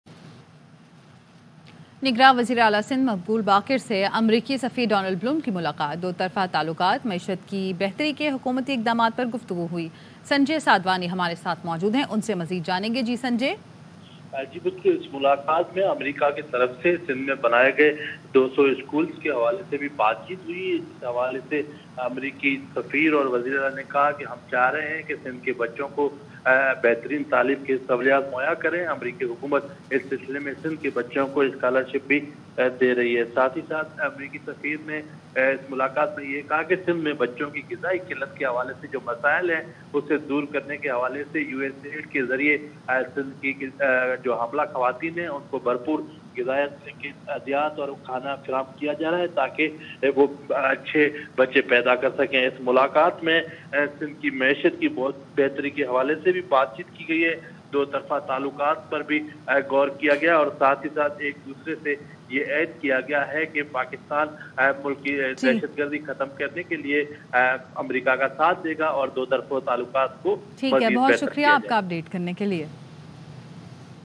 US Ambassador to Pakistan Donald Blome called on Caretaker CM Sindh Maqbool Baqir
KARACHI  -  US Ambassador to Pakistan Donald Blome called on Caretaker Sindh Chief Minister (CM) Justice (retd) Maqbool Baqar in Ka­rachi on Monday. Bilateral relations, steps taken by the government to improve the economy, two hundred schools constructed by the US government in Sindh and other is­sues came under discussion in the meeting.

The Sindh CM said on the occasion that the US is helping the provincial government in education, health and malnutrition besides restoration of flood affected areas.